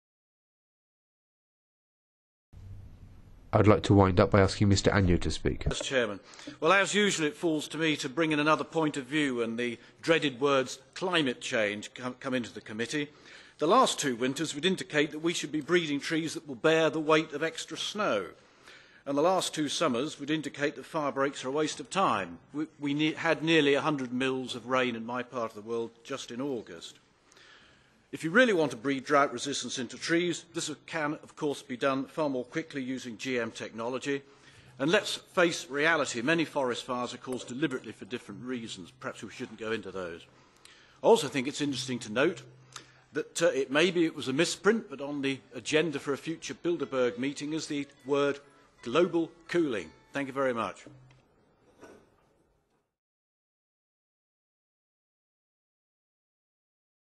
Bilderberg has 'Global Cooling' issue on agenda - Stuart Agnew MEP
► European Parliament, Brussels - 29 September 2010

► Speaker: Stuart Agnew MEP, UKIP (Eastern Counties), EFD group.

- Commission Green Paper "On forest protection and information in the EU: preparing forests for climate change"
(AGRI/7/03333)